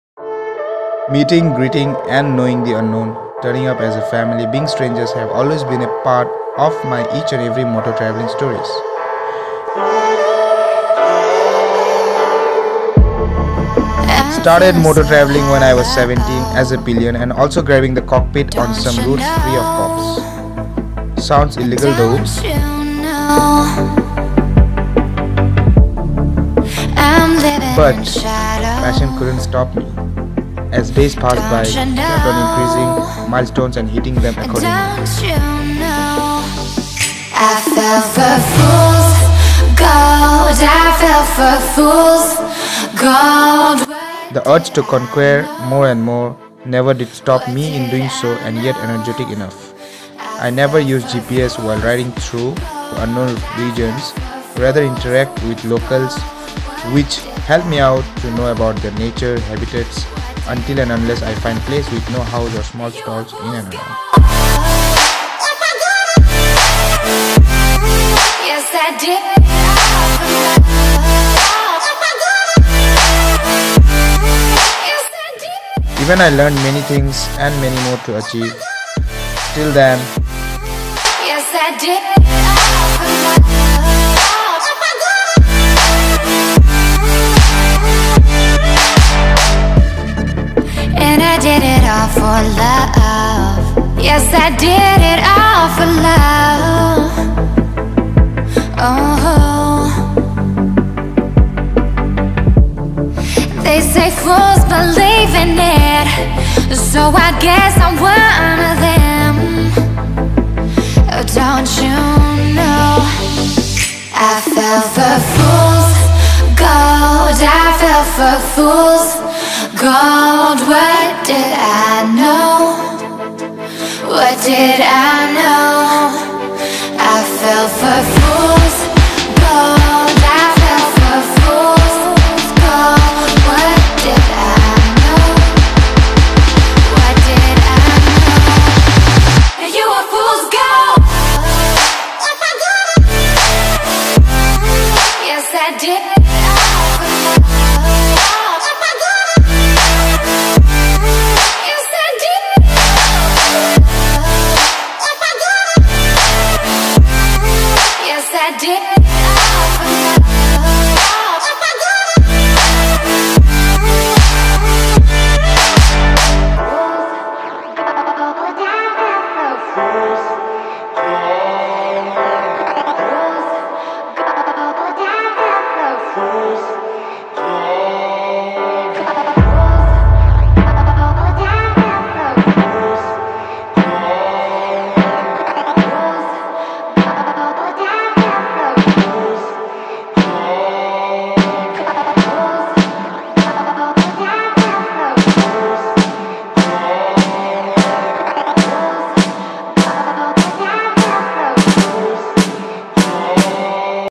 Solo tale about my moto travel till date | NortheastIndia |Vlog- 17
Meeting, Greeting and knowing the unknown , turning up as a family being strangers have always been a part of my each and every Moto travelling stories... Started moto travelling when i was 17 as a pillion and also grabing the cockpit on some routes free of cops.. Sounds illegal though but passion couldn't stop me😅.. As days passed by kept on increasing milestones and hitting them accordingly.. The urge to conquer more and more never did stop me in doing so and yet energetic enough. I never use GPS while riding through unknown regions rather interact with locals which helps me out to know about their nature and habitats untill and unless i find place with no house or small stalls in and around. Learnt many things and many more to achieve... .
Helped by
Vivek debbarma
Motovlog- Hero gopro 6
Dslr:-  Nikon D5500
Drone:- DJI MAVIC Air
 Song used- Helped by
Vivek debbarma